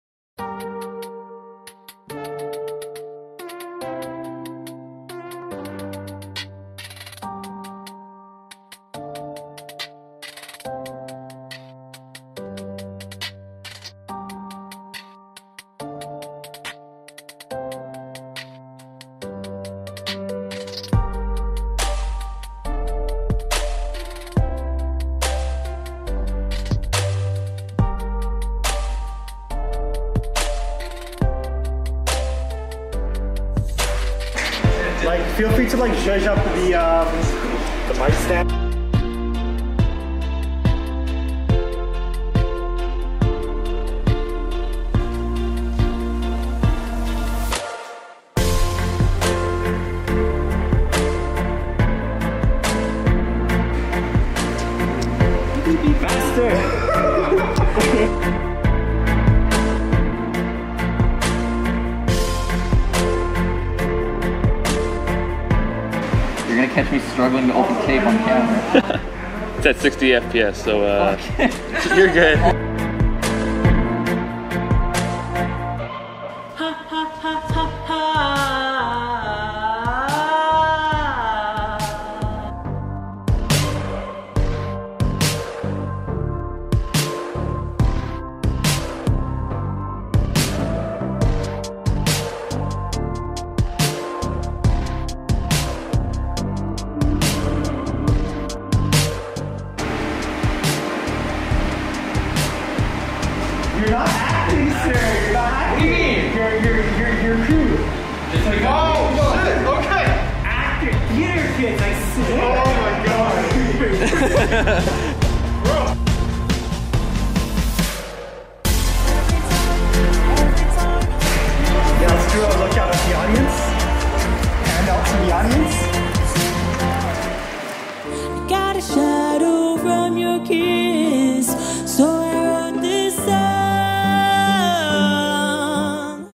Vicki Lovelee - Virtual Concert (Behind The Scenes)
Creating this virtual concert has been my biggest project so far! It was new and challenging but I had a tons of fun and learned so much from my team. I'm super excited to keep making music and I can't wait to share it with you all!

✨ THANK YOU TO MY TEAM:
Co-director/DOP/Producer/Editor: Kevin Young
Co-director/CD/Producer/Stills: Ziyaad Haniff
Producer/CD: Vicki Lovelee
Assistant Director: Tejas Panchal
Camera Assistant: Yusuf Zine
Grip & Electric: Derek Wong
Live Sound Mixing: Darryl Jeremiah
Lighting Design: Ben Poon
Set Dresser: Daniel Nguyen
Mixed & Mastered: Indigo Son
Filmed at: Shakedown Sound & Lighting